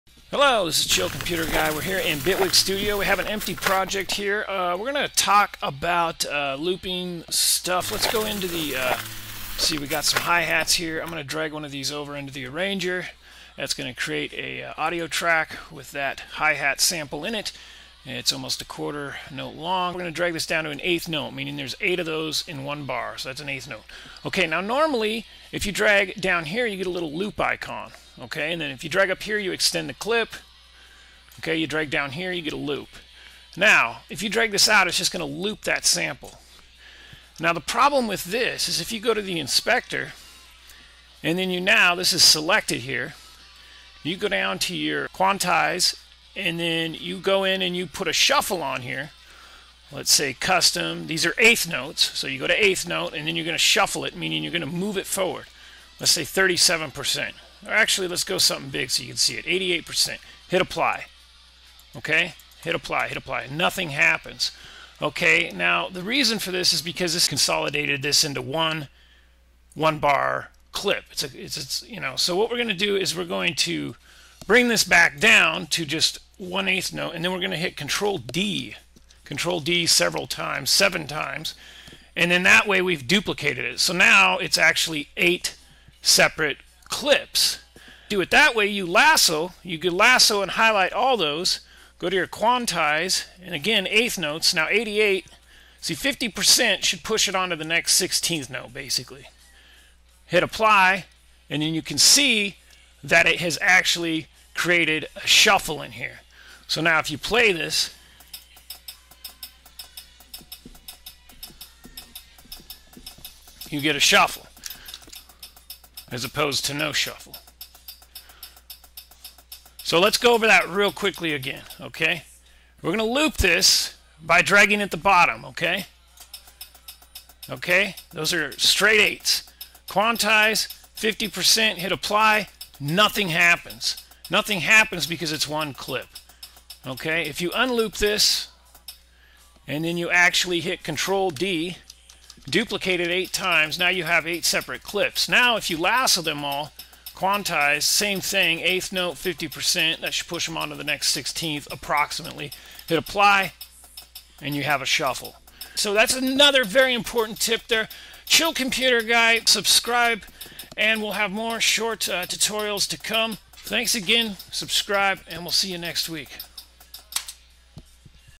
Bitwig Quick Tips: Shuffle part 1
Bigwig studio has a very useful looping function that if used disables the ability to use quantize shuffle. In the short tutorial we will discuss duplicating a clip as opposed to using the loop feature, as well as some basic shuffle info.